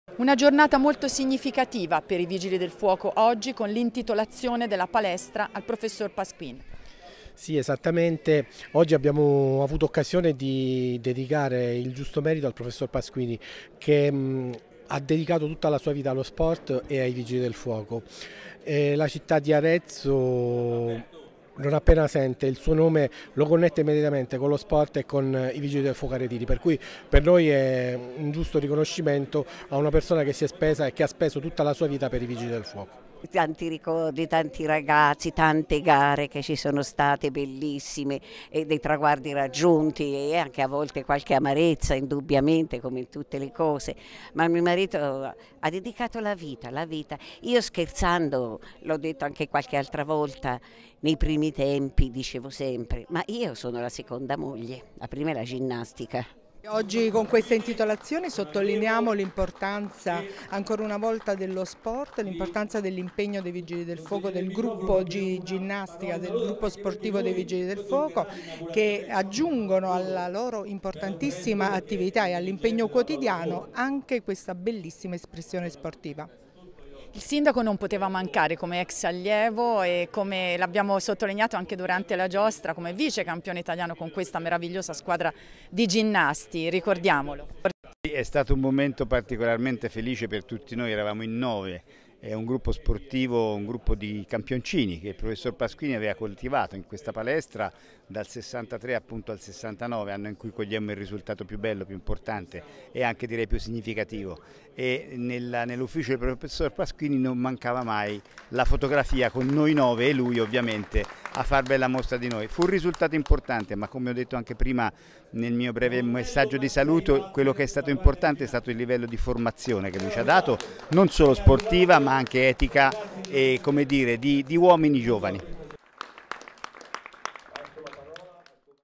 Palestra dei VVf intitolata al prof Pasquini
Con una bella cerimonia la palestra dei vigili del fuoco di Arezzo è stata intitolata al professor Giuseppe Pasquini, per anni alla guida del gruppo sportivo. Oggi sarebbe stato il suo 89esimo compleanno.